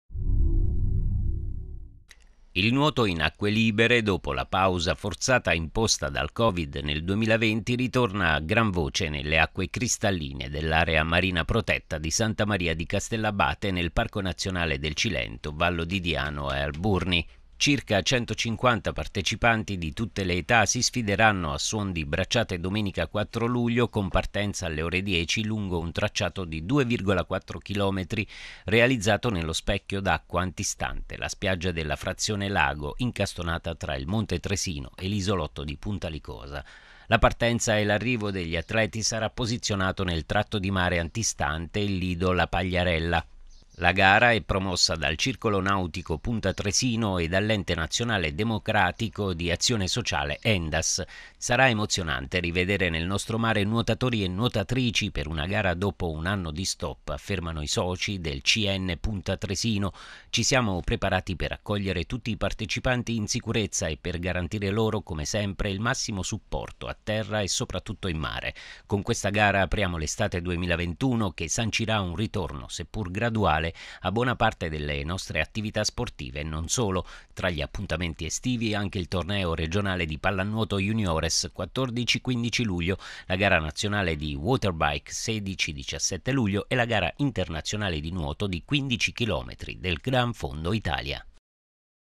Castellabate, Il nuoto in acque libere ritorna nell’area marina protetta
Il nuoto in acque libere, dopo la pausa forzata imposta dal Covid nel 2020, ritorna a gran voce nelle acque cristalline dell’Area Marina Protetta di Santa Maria di Castellabate, nel Parco Nazionale del Cilento, Vallo di Diano e Alburni.